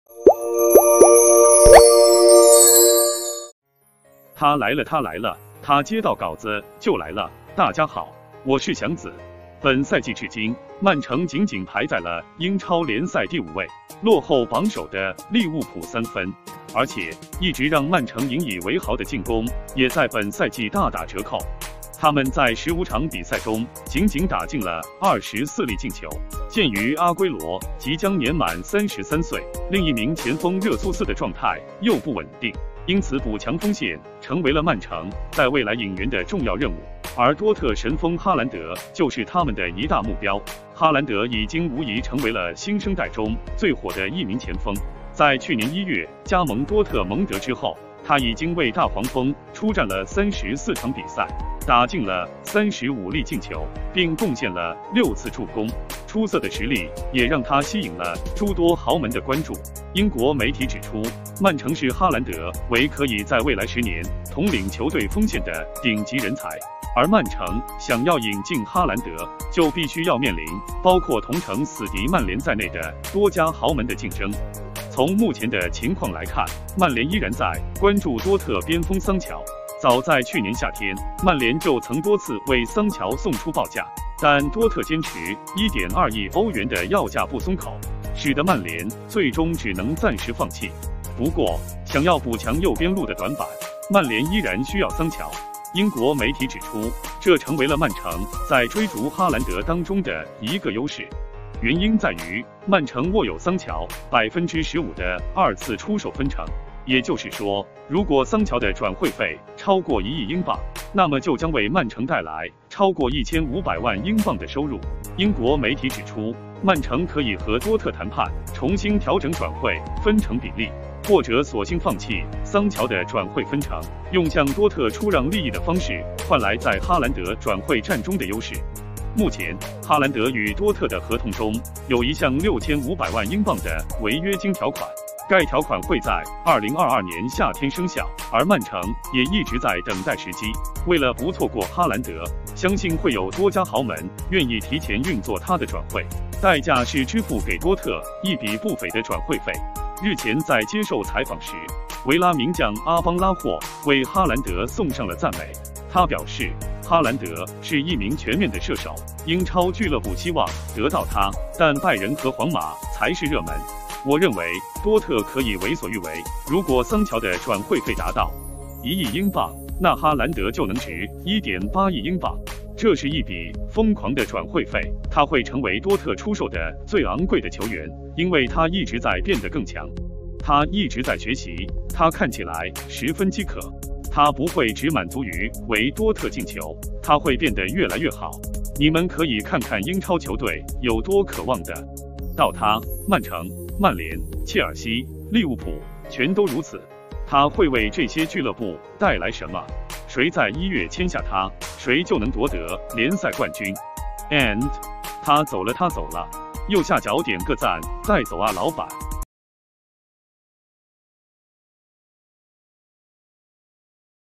曼城未来10年的目标取决于他！哈兰德签约策略曝光后，桑乔成了工具人
本赛季至今，曼城仅仅排在了英超联赛第5位，落后榜首的利物浦3分。而且，一直让曼城引以为豪的进攻也在本赛季大打折扣，他们在15场比赛中仅仅打进了24粒进球。